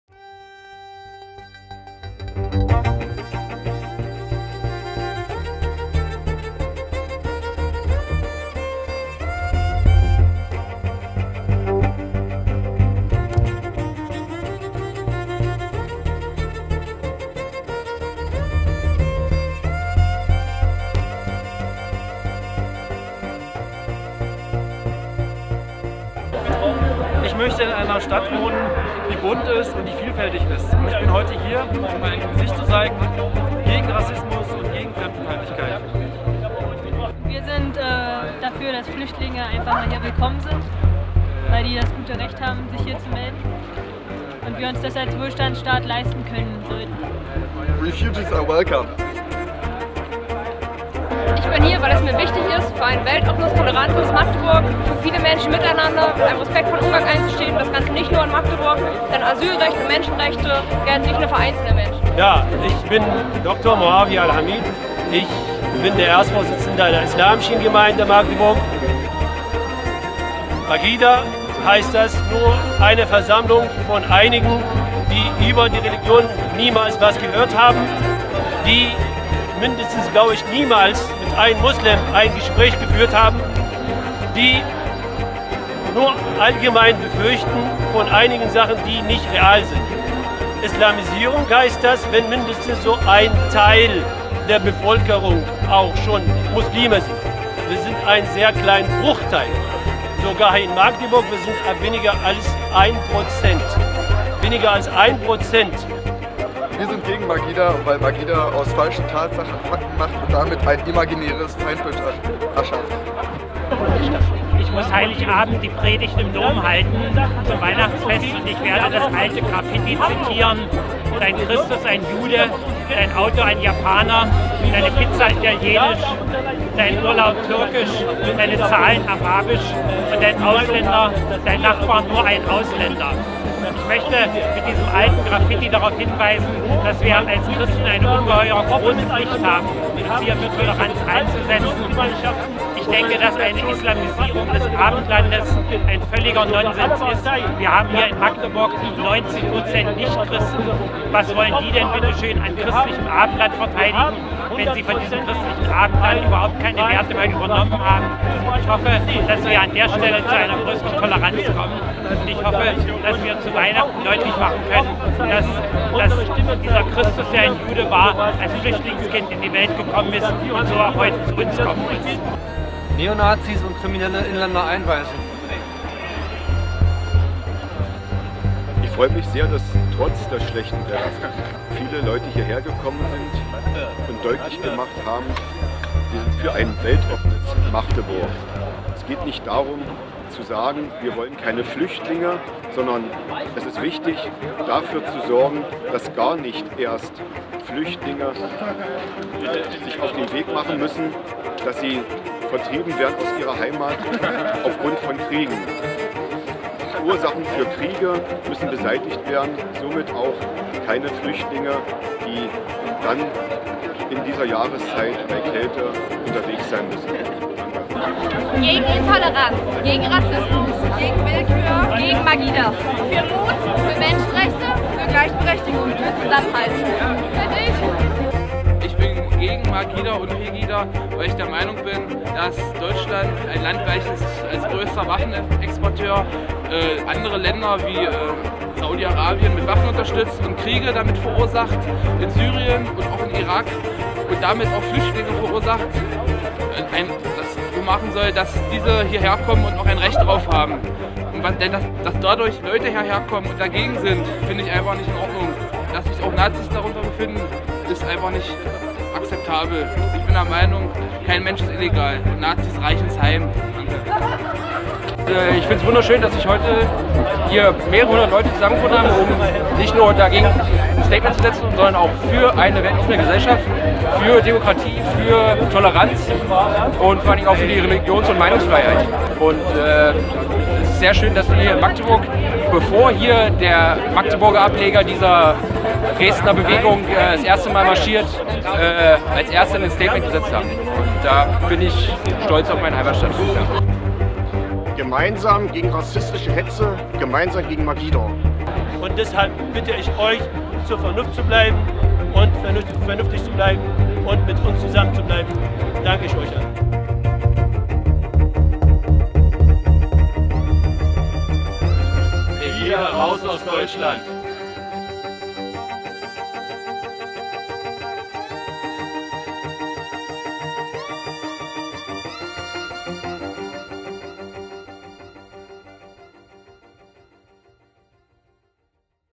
NoMAGIDA
Spontane Statements, aufgezeichnet bei der Kundgebung "Das Problem heißt Rassismus" auf dem Magdeburger Domplatz am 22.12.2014.